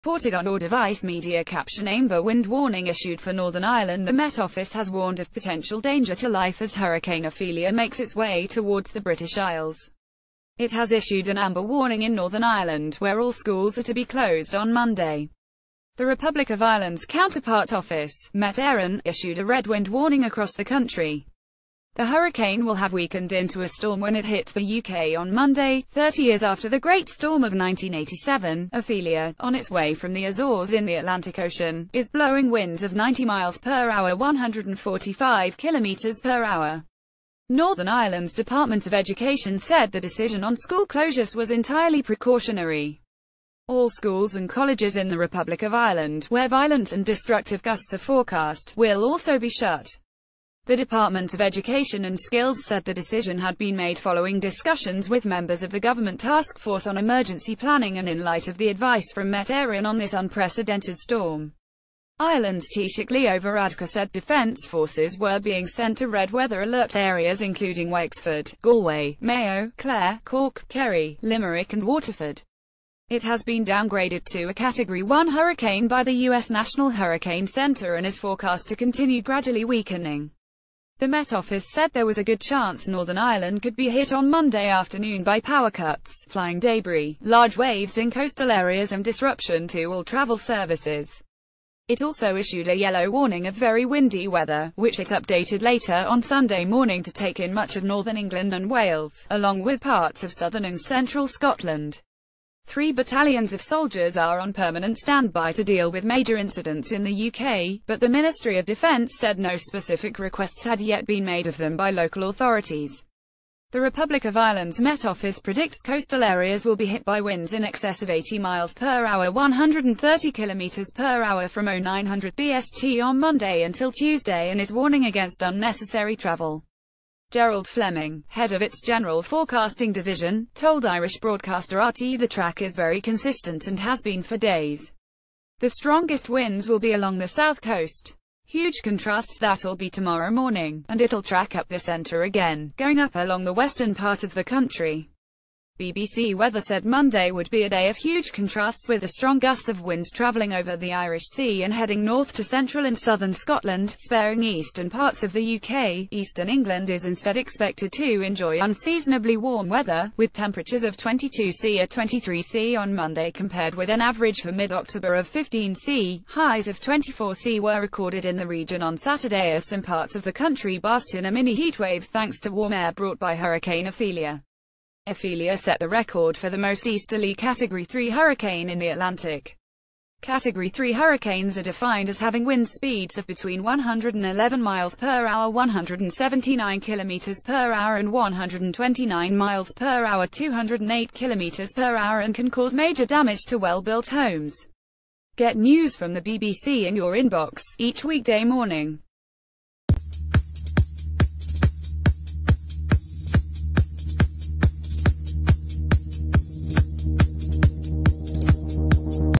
Breaking News: Red Alert; Hurricane Ophelia Danger to life' warning as storm heads to UK
Danger to life' warning as storm heads to UK. The Met Office has warned of "potential danger to life" as Hurricane Ophelia makes its way towards the British Isles.
It has issued an amber warning in Northern Ireland, saying power cuts and disruption to transport are possible.
The Republic of Ireland's counterpart office, Met Eireann, issued a red wind warning across the country.
The hurricane will have weakened into a storm when it hits the UK on Monday, 30 years after the Great Storm of 1987.
Ophelia, on its way from the Azores in the Atlantic Ocean, is blowing winds of 90mph (145km/h).
All schools and colleges in the Republic of Ireland, where "violent and destructive gusts" are forecast, will be closed on Monday.
The Department of Education and Skills said the decision had been made "following discussions with members of the Government Task Force on Emergency Planning and in light of the advice from Met Eireann on this unprecedented storm."
Ireland's Taoiseach Leo Varadkar said defence forces were being sent to red weather alert areas - including Wexford, Galway, Mayo, Clare, Cork, Kerry, Limerick and Waterford.